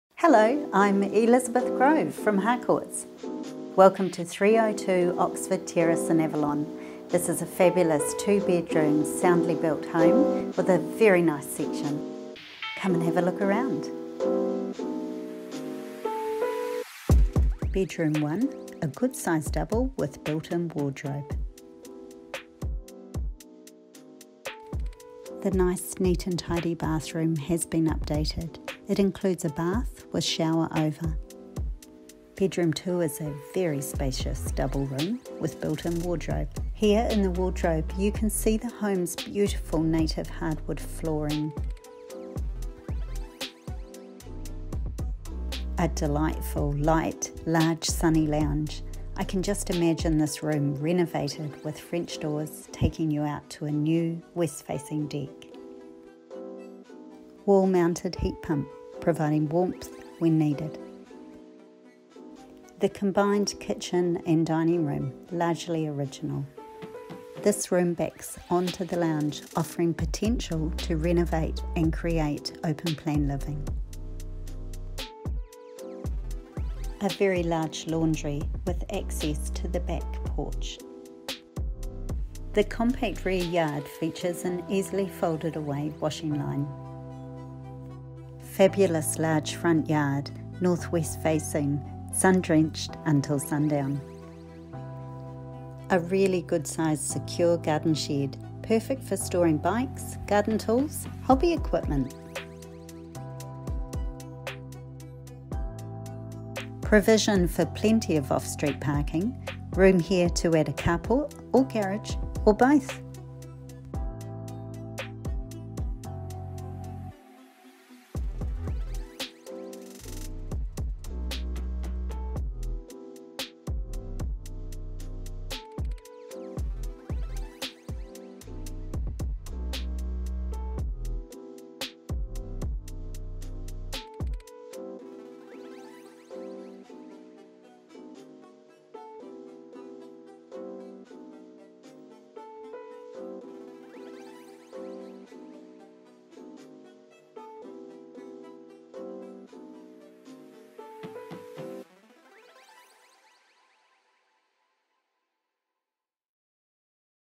302 Oxford Terrace, Avalon, Lower Hutt
Delightful soundly built 1950s two bedroom, one bathroom home in great Avalon location. Perfectly sited for sun; handy to shops, schools and public transport. Ready to move in and enjoy as is, this home offers plenty of potential to add value through your own renovation ideas! An excellent opportunity for First Home Buyers, Investors, and DIYers looking for a project to keep busy with. Deadline Sale closes 4:00pm Thursday 21 May 2020 (unless sold prior). Elizabeth Grove, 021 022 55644 or 0800 NEXT HOME